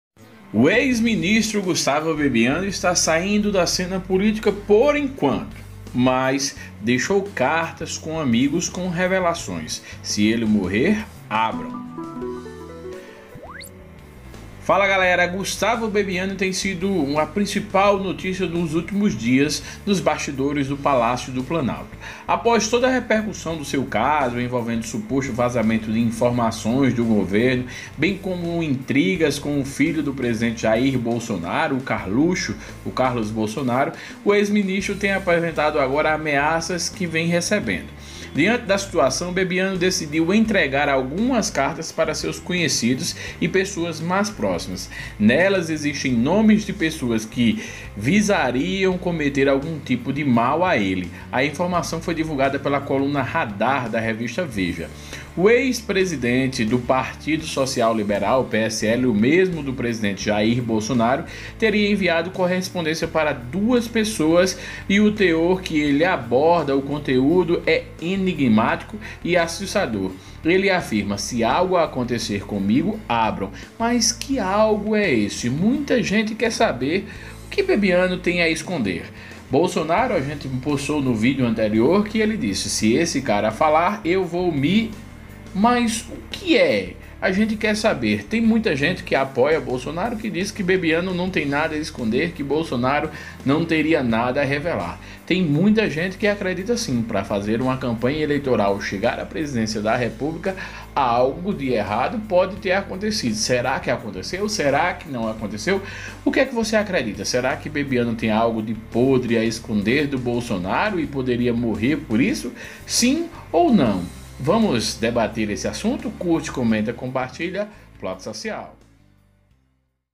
BEBIANNO ENTREGA CARTAS A AMIGOS: "SO ABRAM SE ALGO RUIM ACONTECER COMIGO"
Gustavo Bebianno tem sido a principal notícias dos últimos dias nos bastidores do Palácio do Planalto. Após toda a repercussão de seu caso, envolvendo supostos vazamentos de informações do governo, bem como intrigas com o filho do presidente Jair Bolsonaro, o ex-ministro tem apresentado agora as ameaças que vem recebendo.

Diante da situação, Bebianno decidiu entregar algumas cartas para os seus conhecidos e pessoas mais próximas. Nelas, existem os nomes de pessoas que visariam cometer algum tipo de mal a ele. A informação foi divulgada pela coluna Radar, da Veja.

O ex-presidente do Partido Social Liberal (PSL), o mesmo de Jair Bolsonaro, teria enviado correspondências a duas pessoas, e o teor com que ele aborda o conteúdo é enigmático e assustador. Ele afirma: “Se algo acontecer comigo, abram”, escreveu Bebianno.

As supostas ameaças que ele teria recebido, chegaram via aplicativo de mensagens instantâneas WhatasApp, após ter o seu número de telefone divulgado.

O ex-Secretário-Geral informou a pelo menos dois de seus amigos mais próximos que estava recebendo este tipo de conteúdo em seu celular. Os episódios tiveram início, segundo ele, no domingo (17).  A Folha divulgou que Bebianno deve denunciar o ocorrido às autoridades.

A divulgação de seu número pessoal teve início após o surgimento da polêmica.

Entretanto, um dos interlocutores de Bebianno afirma que essas mensagens são, em grande parte, dos apoiadores mais radicais do presidente, e que não expressariam nenhuma ameaça mais preocupante. Todavia, todo o cuidado é pouco diante de tais situações.